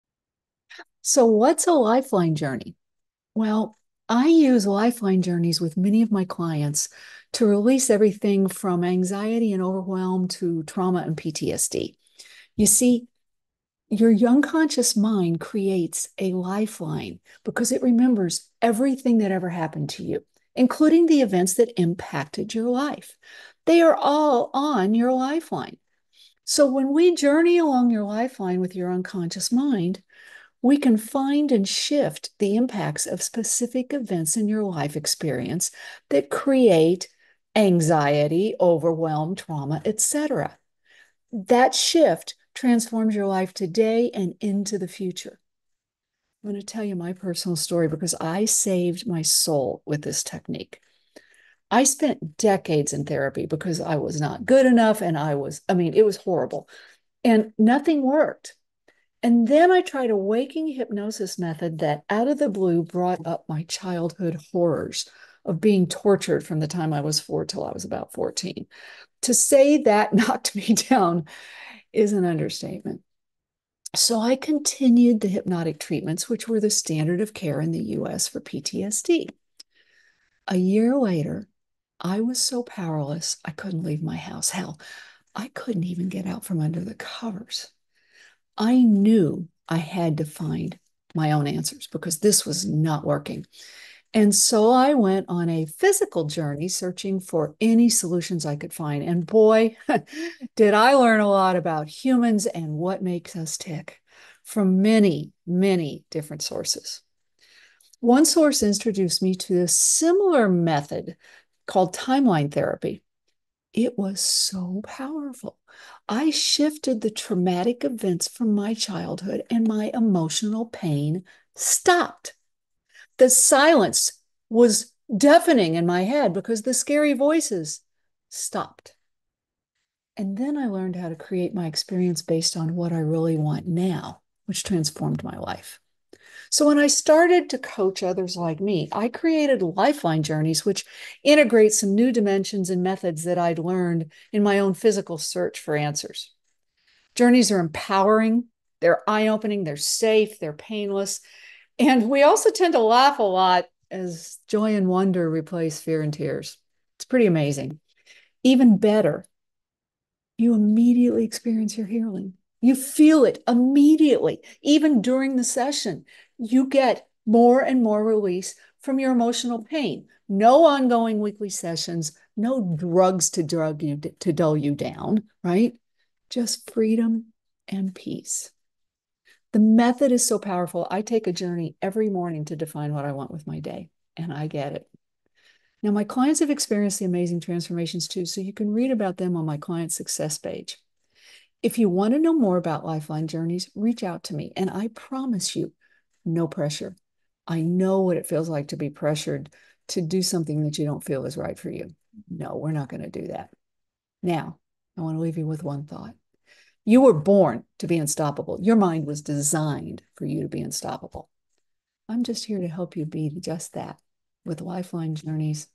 Quantum Journeys
We exist in a quantum field, filled with limitless possibilities. Our mind selects which possibilities we experience based on what it thinks we want. When we journey into our quantum world, we can consciously teach our unconscious mind exactly what we want to experience in our future. We can also shift our perceptions and beliefs from our past, empower our present and future beyond limitations.
In personal, private, 1-1 guided journeys, we explore your quantum lifeline; past, present, and, yes, your future. We communicate with your unconscious mind, using the sensory insights it needs to truly understand what you want. Your unconscious mind then does what it is designed to do…  use your quantum lifeline and field to shift your present and your future in the direction you desire.